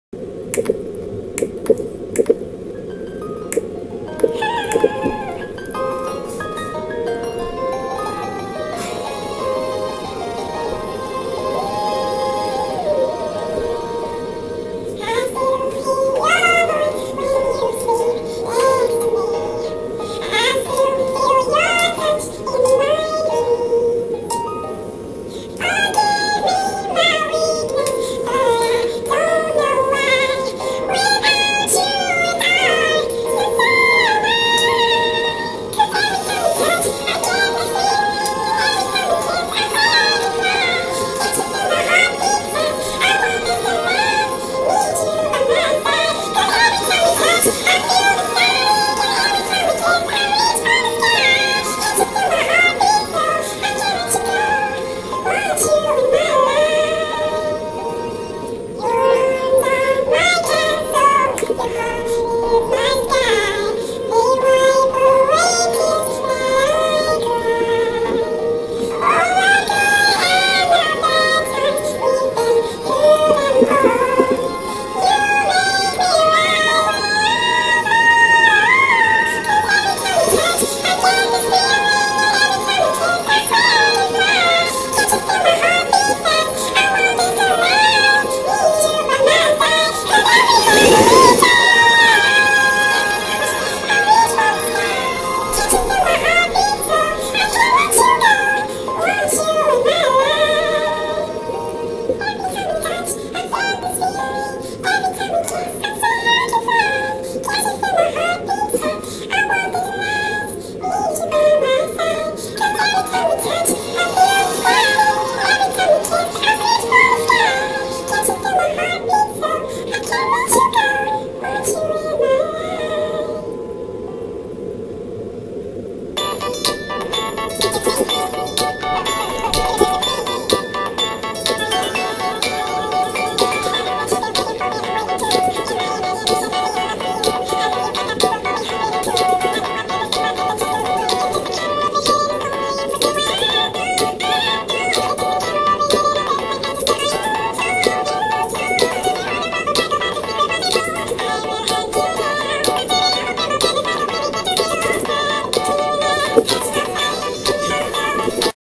RANDOM LIPSORTOFSINGING o-x
lfmsjdfiogkdhd i dunno i was bored while straightening my haaaaaaaaair be jealous
DONT BE MEAN EITHER,